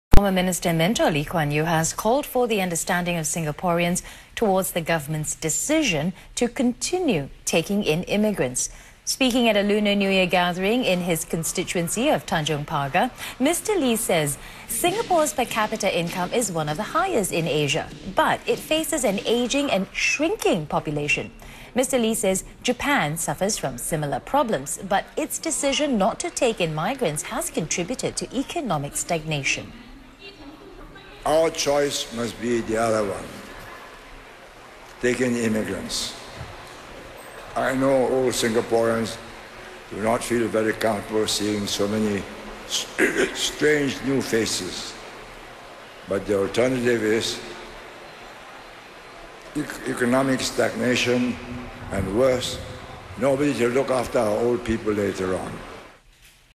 Lee Kuan Yew calls for understanding towards immigration policy
SINGAPORE: Former Minister Mentor Lee Kuan Yew has called for the understanding of Singaporeans towards the government's decision to continue taking in immigrants. First published Feb 3 2012.